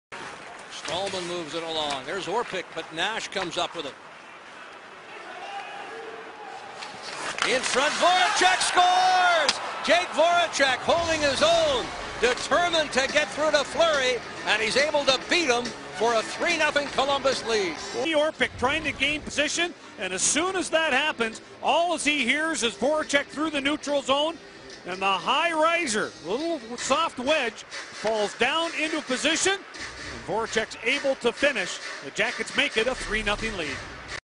Rick Nash Airmail Assist to Jakub Voracek 2/8/11 @ Pittsburgh
Rick Nash makes a perfect airmail pass to Jakub Voracek who beats Marc-Andre Fleury.